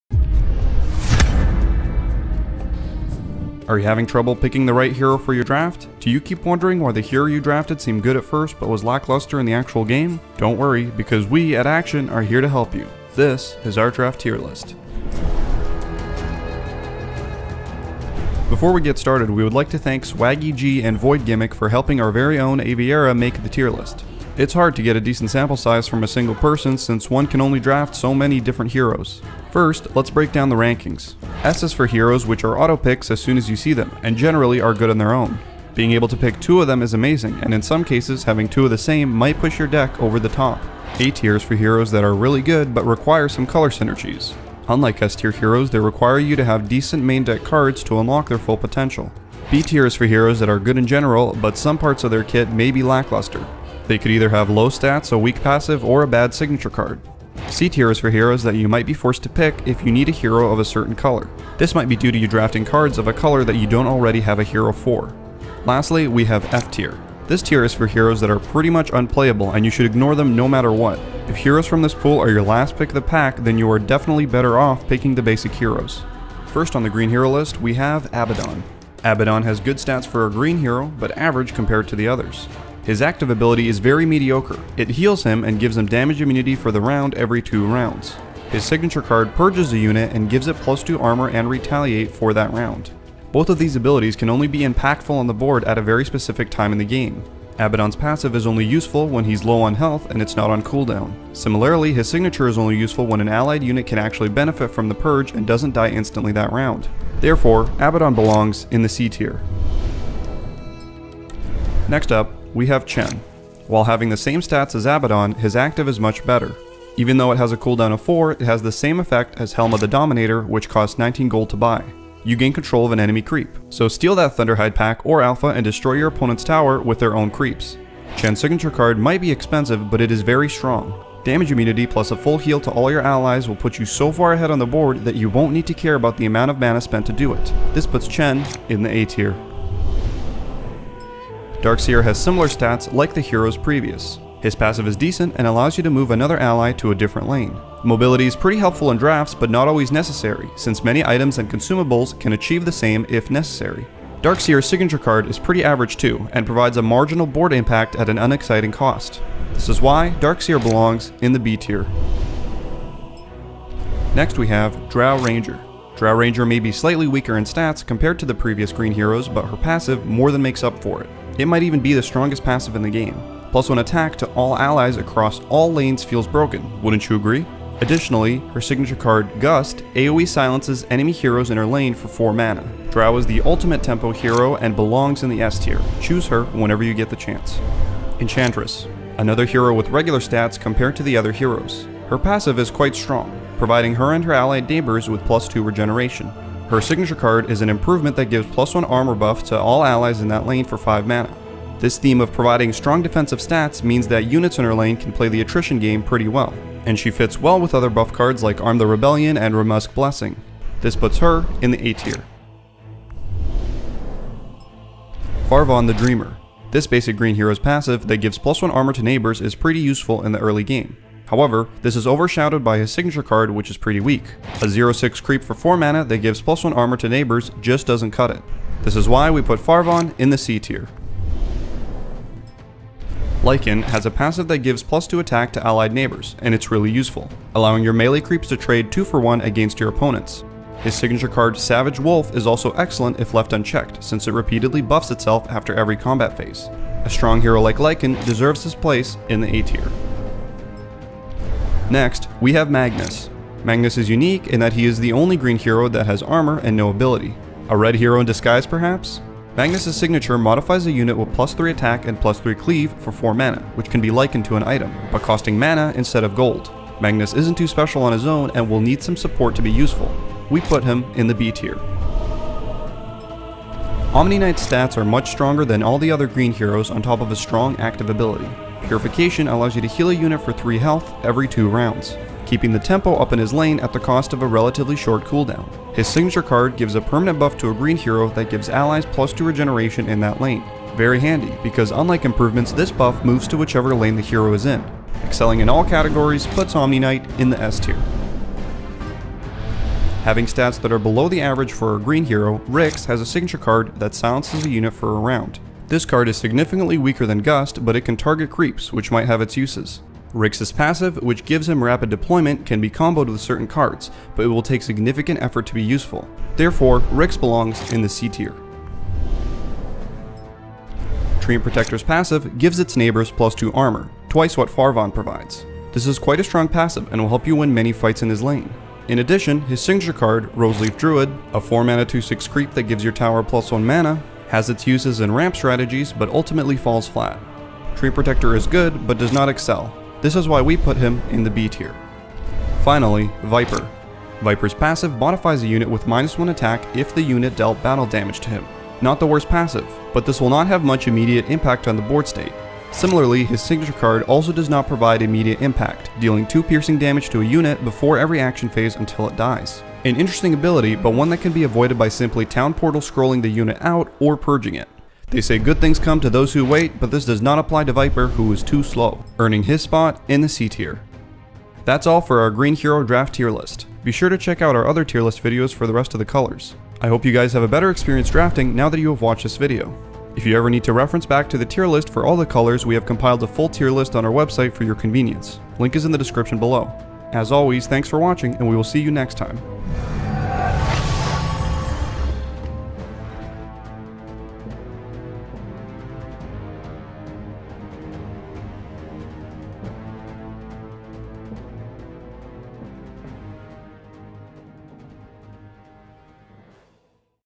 Artifact: Green Hero Build 1.0 Draft Tier List - Akshon Esports
In today's Artifact video, we put the various Green heroes from the base set and Call To Arms in our Tier list to help players make more informed decisions when it comes to constructing a deck in Draft.

Compared to their more aggressive and combat oriented Red counterparts, Green heroes in Artifact are designed around a more supportive playstyle with abilities focusing on summons, healing, or buffs. While slow to start off, once given enough time, decks that make use of Green heroes and other cards in this house are able to quickly overpower an enemy by the mid to late game if left unchecked.